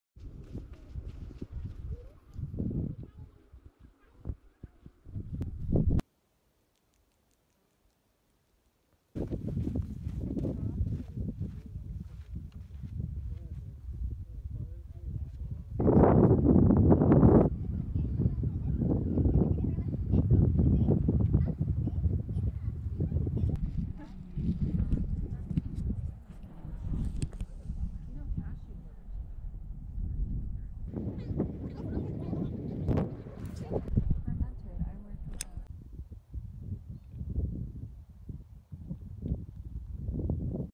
Mt Rainier.
Views of Mt Rainier from the trails around Sunrise hiking area.